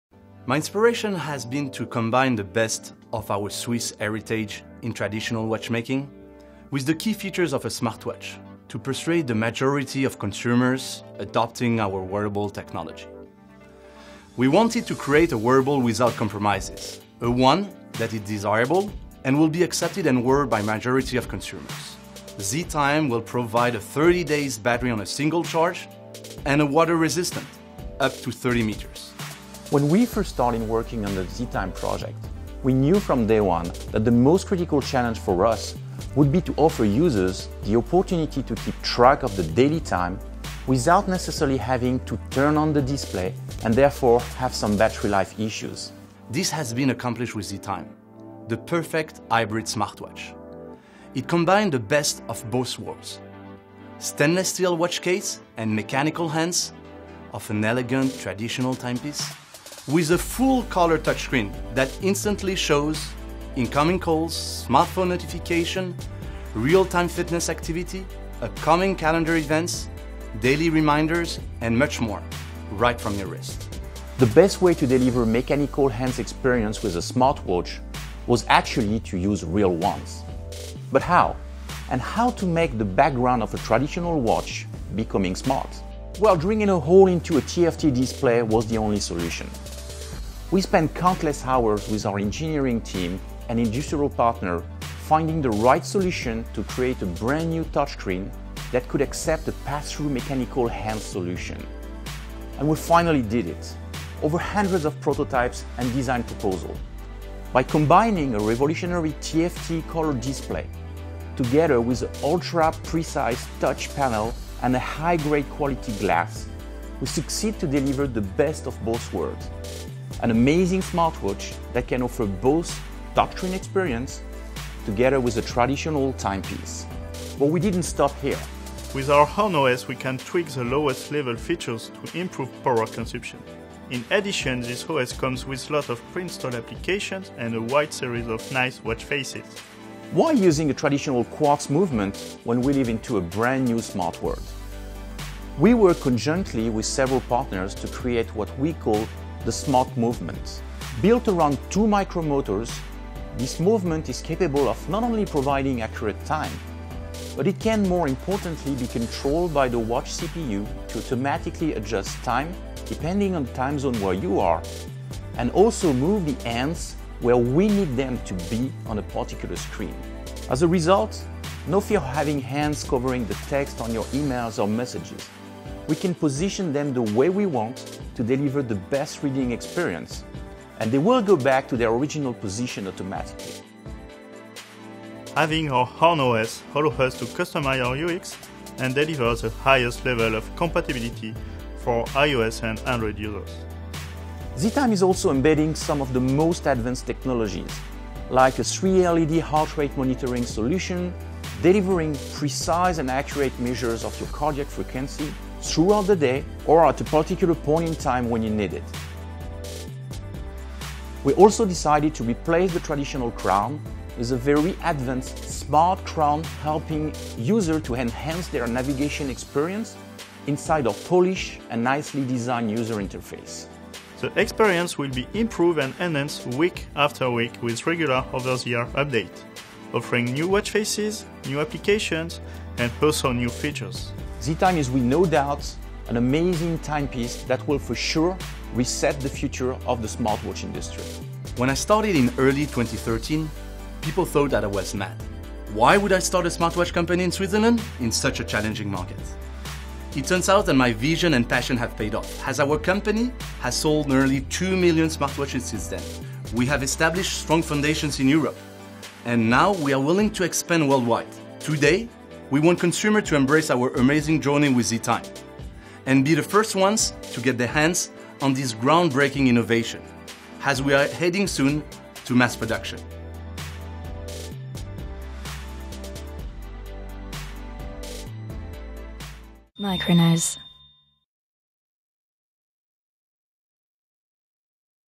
ZeTime: World's first SMARTWATCH with HANDS OVER touchscreen
ZeTime: the world’s first hybrid smartwatch combining mechanical hands with a full round color touchscreen. Using groundbreaking technology to mount watch hands through the center of the high-resolution TFT color display, ZeTime blends the classic design of a traditional timepiece with the most advanced features of a smartwatch.